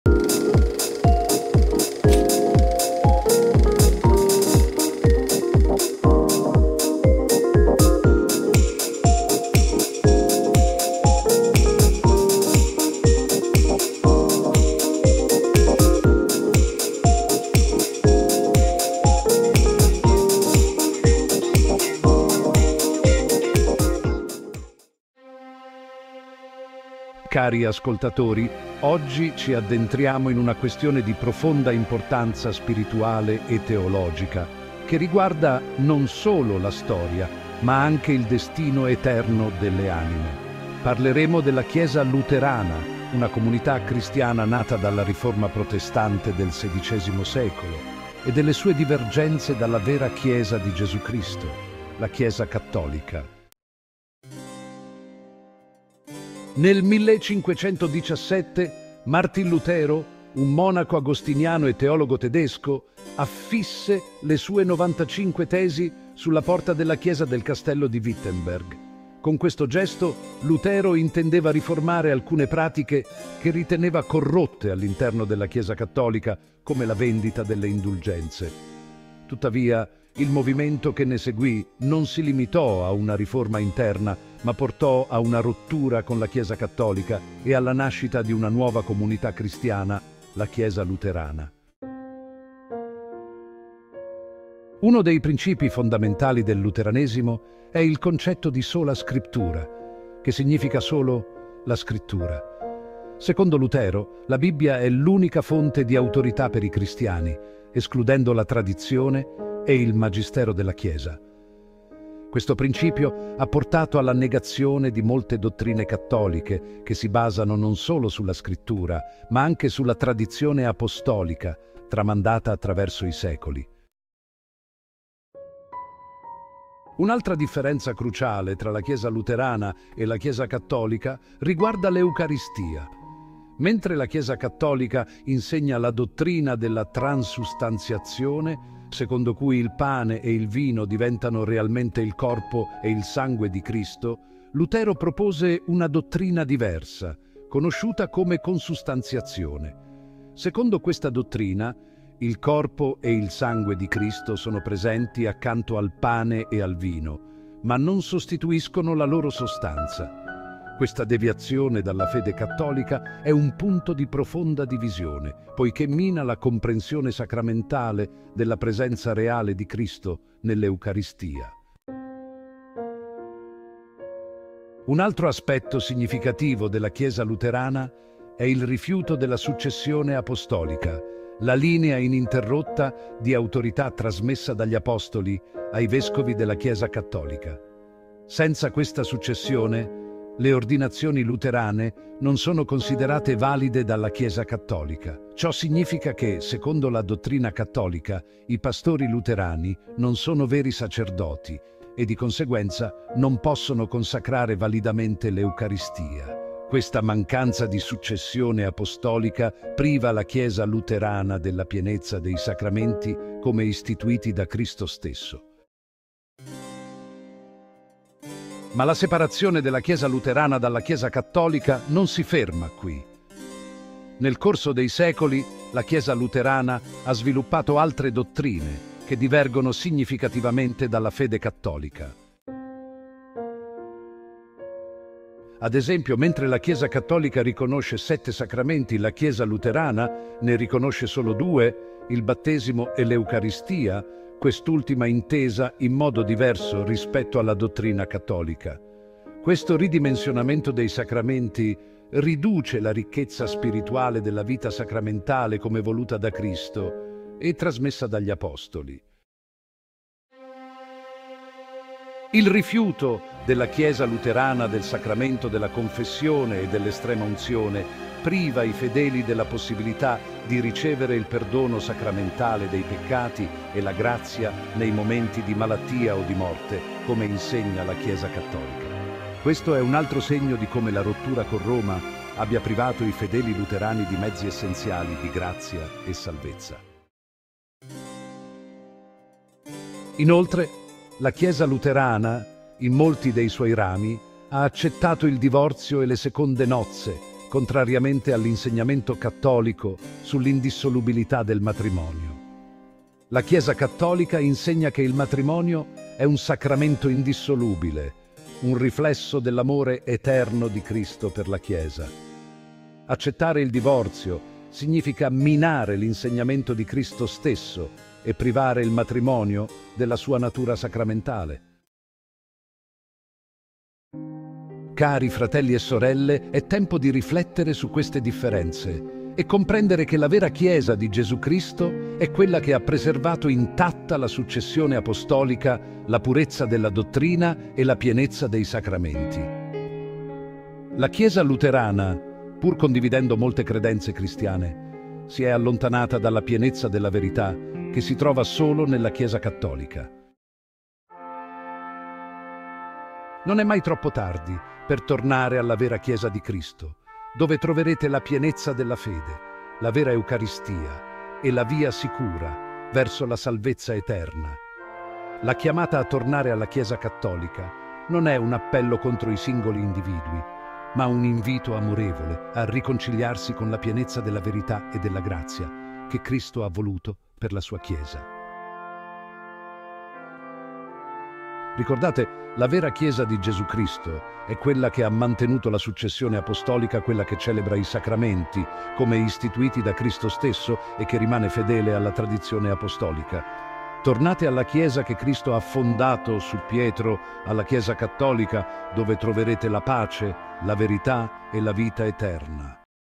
La Verità sulla Chiesa Luterana: Dottrine, Errori e la Vera Chiesa di Gesù Cristo
In questo video esploriamo la nascita e le dottrine della Chiesa Luterana, una delle più influenti tradizioni protestanti fondata da Martin Lutero nel XVI secolo. Analizziamo in profondità le differenze tra la Chiesa Luterana e la Chiesa Cattolica, concentrandoci sulla successione apostolica, la dottrina della predestinazione e la comprensione dell'Eucaristia. Scoprirete perché, secondo la dottrina cattolica, la Chiesa Luterana ha perso la pienezza dei sacramenti e della verità che si trovano solo nella Chiesa Cattolica, e perché è fondamentale tornare alla vera Chiesa fondata da Gesù Cristo.